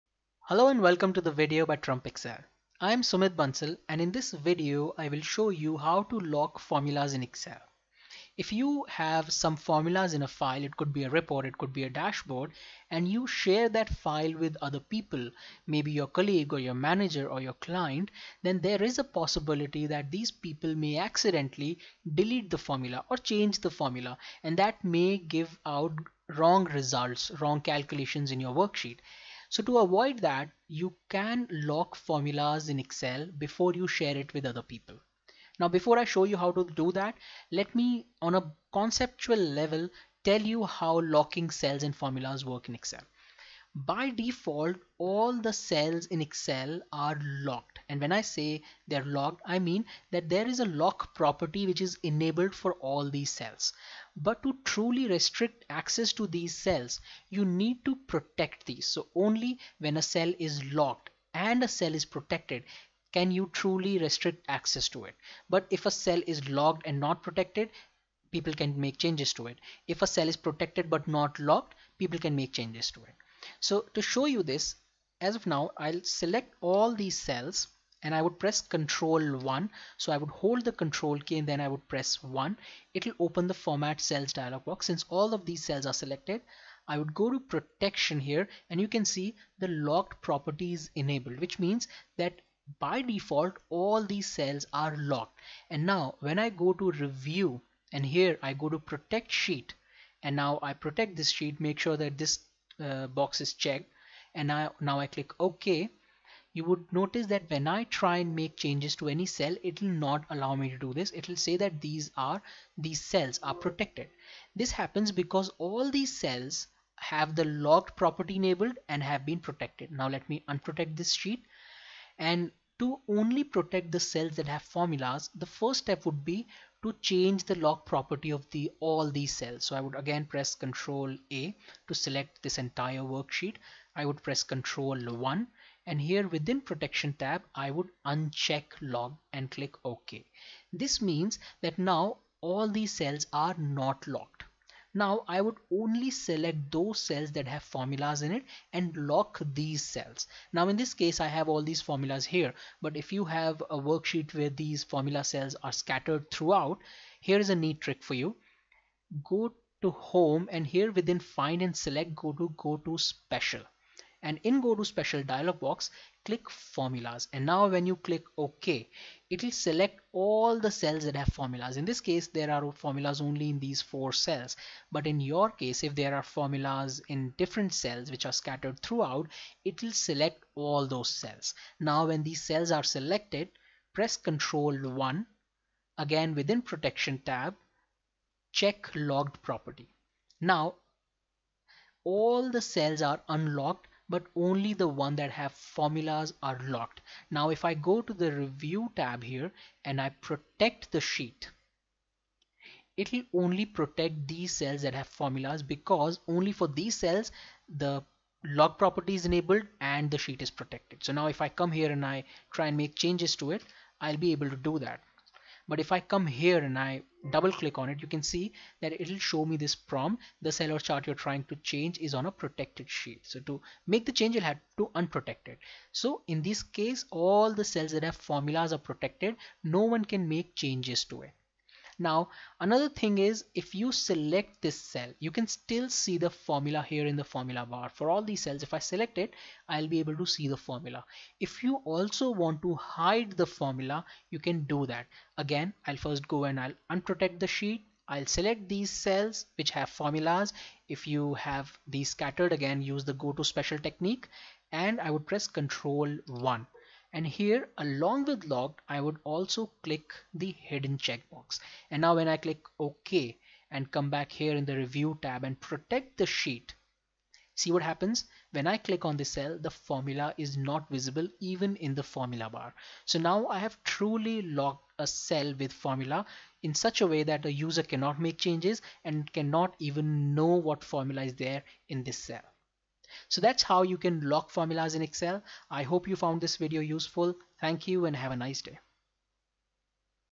Lock Cells that have Formulas in Excel (Easy Steps)
In this video, you will learn how to lock cells that have formulas in it in Excel.

Excel formulas are easy to create and edit in Excel.

You can easily edit a formula in a cell through the formula bar or directly in the cell.

While this makes it convenient to create formulas in Excel, it comes with a few disadvantages as well.

Consider this.

You are going through a worksheet full of formulas, and you accidentally hit the delete key or backspace key, or some other number/alphabet key.

Now you'll be lucky if you're able to spot the error and correct it. But if you don't. it may lead to some erroneous results.

And let me tell you this, errors in Excel have cost millions to companies (read this or this).

The chances of such errors increase multifold when you share a file with colleagues or managers, or clients.

One of the ways to prevent this to lock the worksheet, which in turn lock all the cells in the worksheet. However, doing this would prevent the user from making any changes to the worksheet. For example, if you're sending a workbook to your manager for review, you may want to allow him to add his comments or color code cells as his/her feedback.

A better workaround to this is to lock cells that have formulas in Excel.

In this video, you will learn how to lock cells that have formulas in it.

00:00 Intro
00:35 How Locking Cells Work in Excel
02:01 Protect Cells with Formulas

Also, I have made all of my Excel courses available for free. You can check these out using the below links: